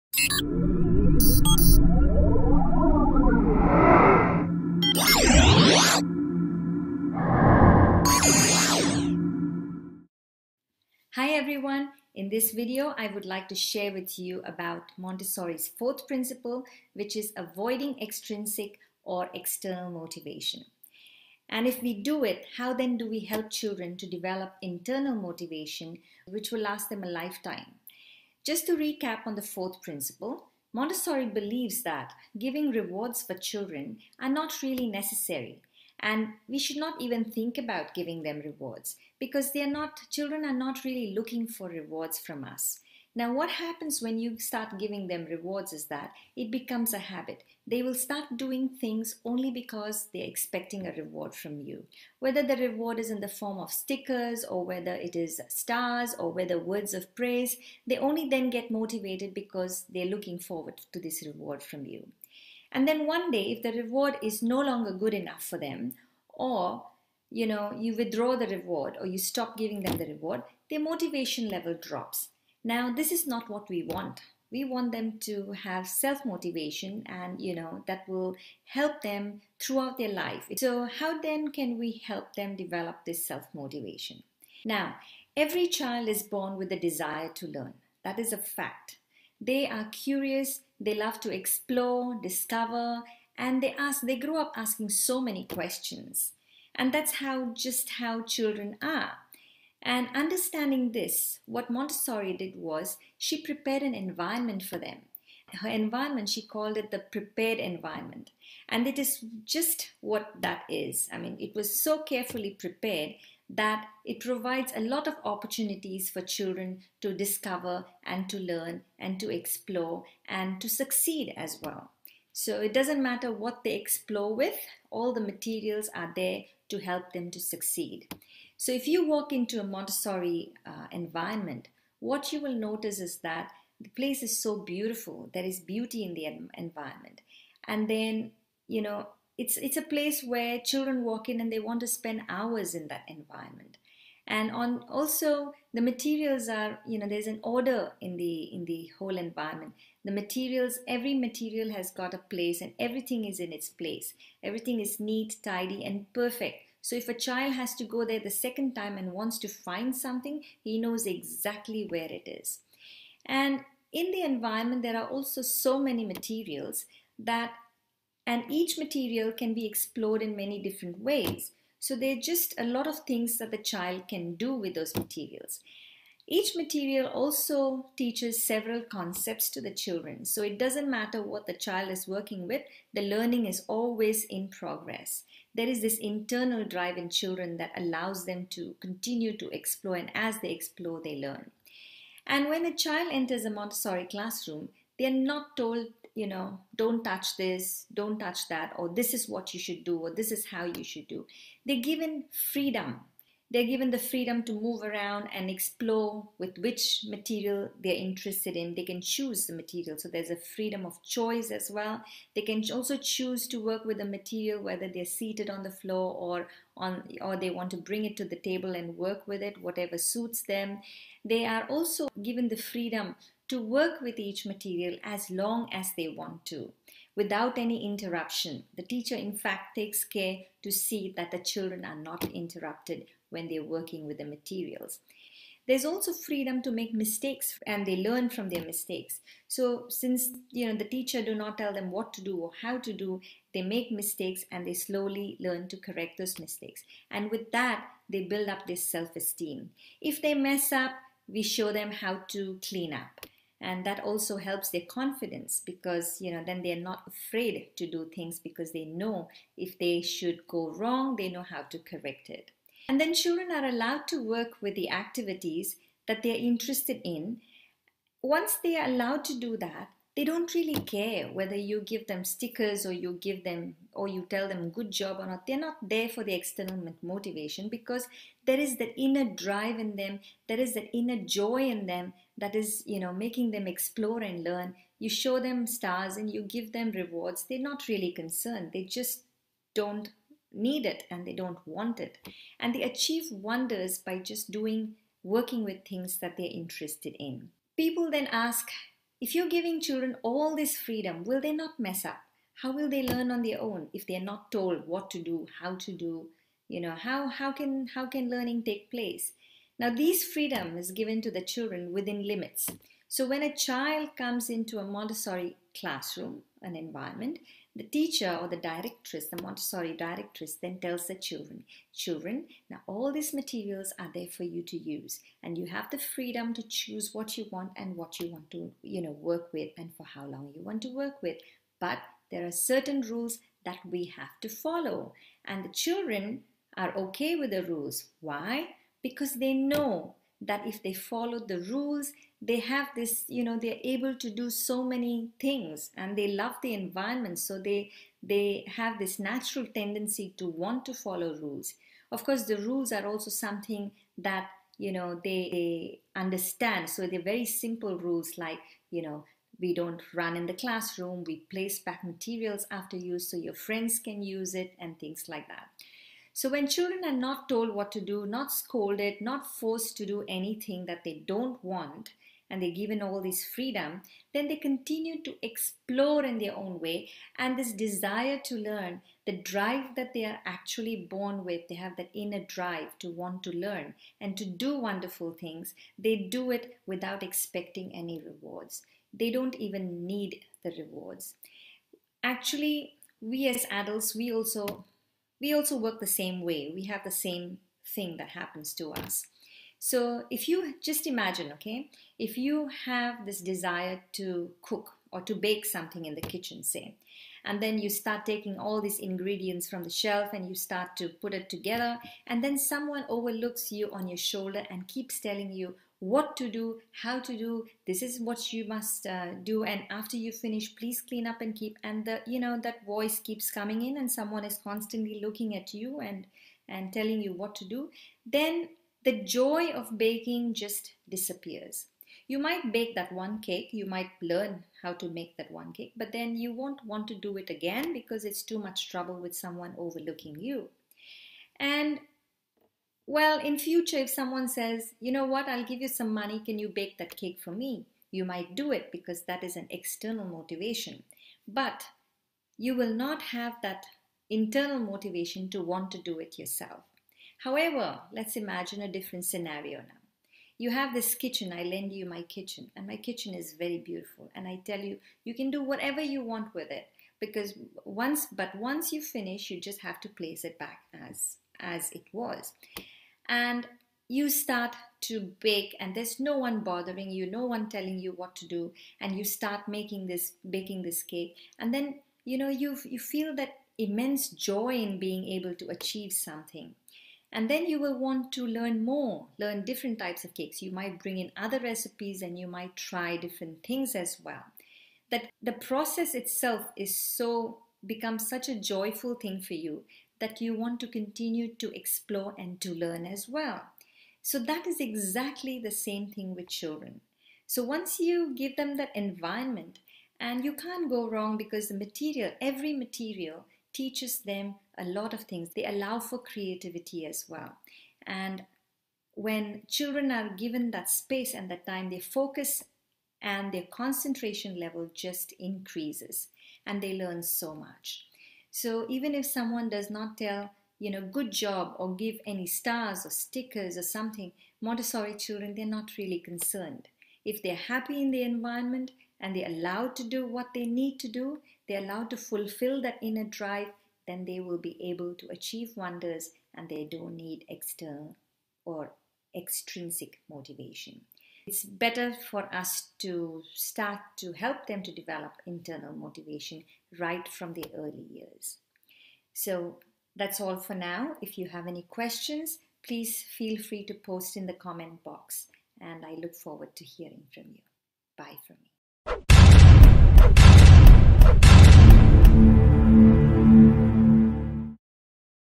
How to Motivate Your Child - Montessori Method
Sony Vasandani speaking about motivating your child the Montessori method

Credits to Sunshine Teachers Training